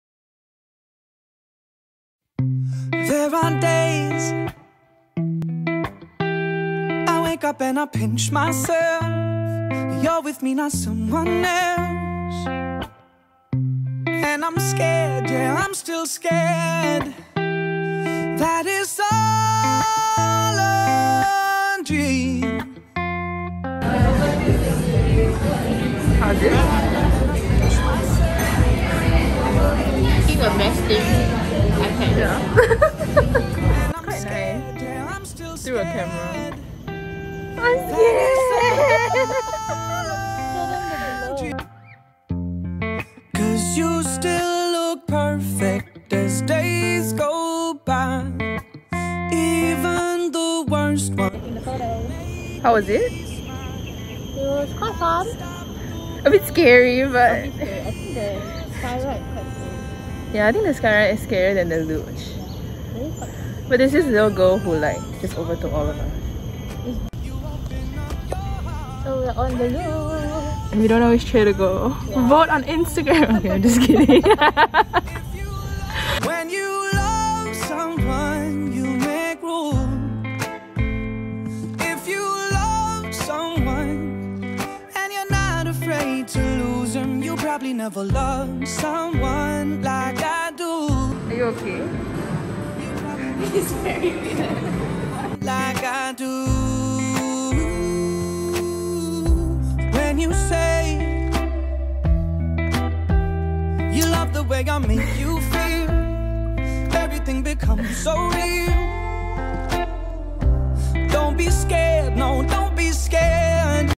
Vlog #3 Luge First Experience | Kream & Kensho Review | Bye Maryam
highly recommended: luge @ sentosa (use your singapoREdiscover vouchers!!) and grape ricotta brioche from kream & kensho